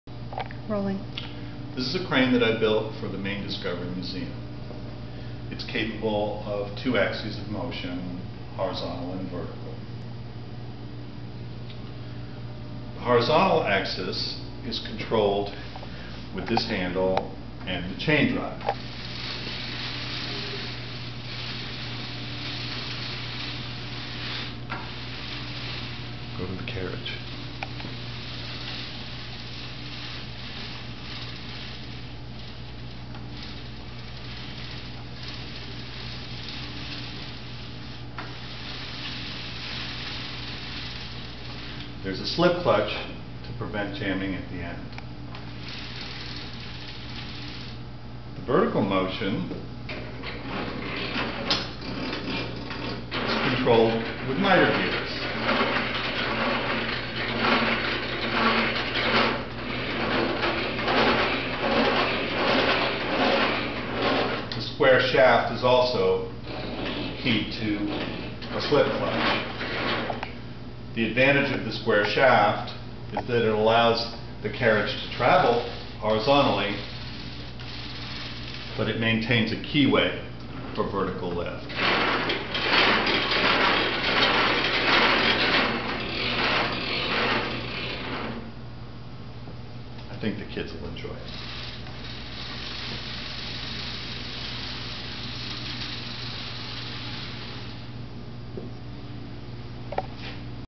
interactive crane exhibit
working model of a boom crane for
children's museum in Bangor Maine